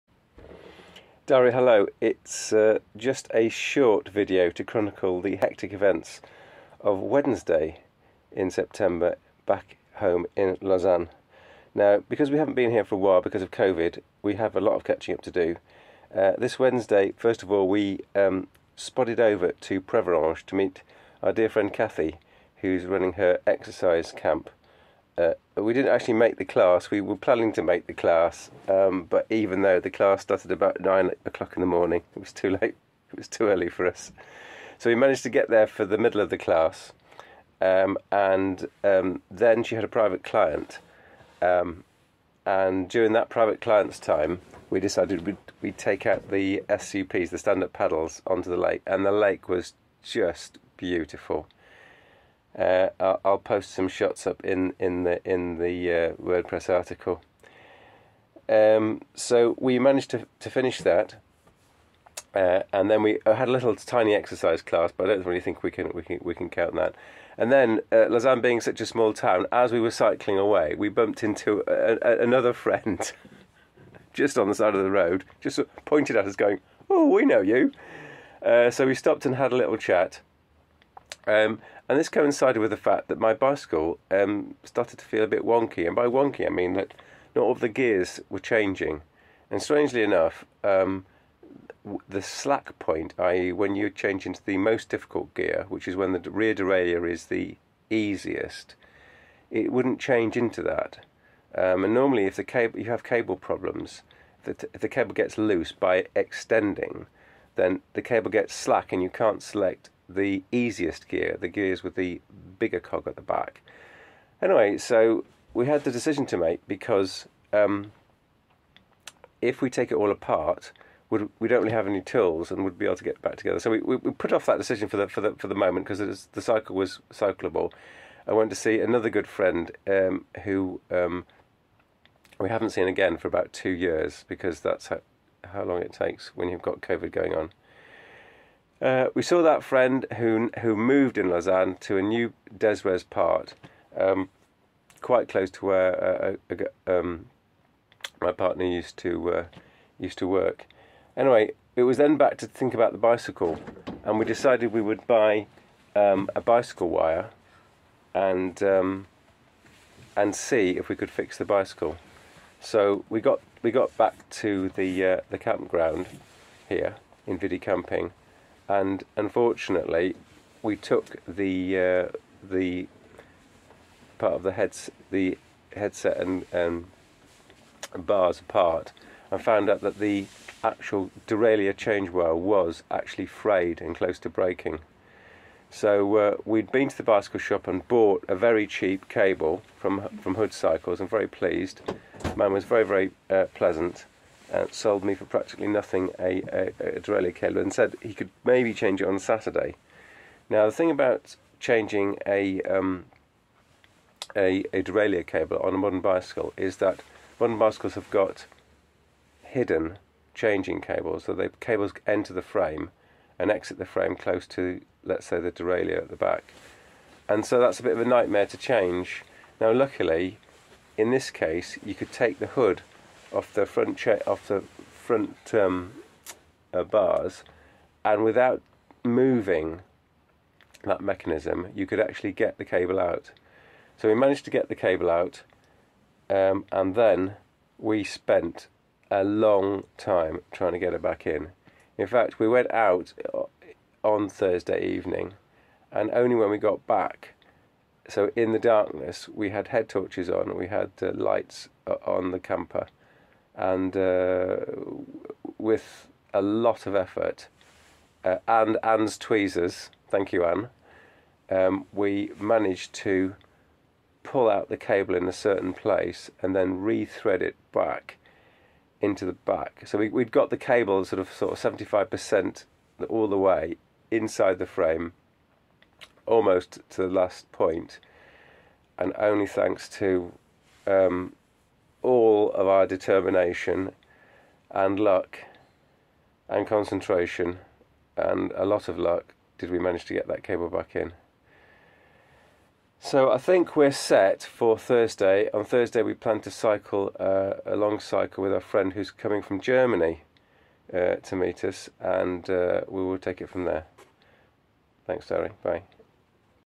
20210908 Wednesday: Lausanne Diary: Exercise and Friends
Preveranges Exercise Class

SUP at Preveranges

First Bicycle wonk signs

Meeting Medalit

First attempt Bicycle Repair

Meeting Ann and Saffy

Second attempt Bicycle Repair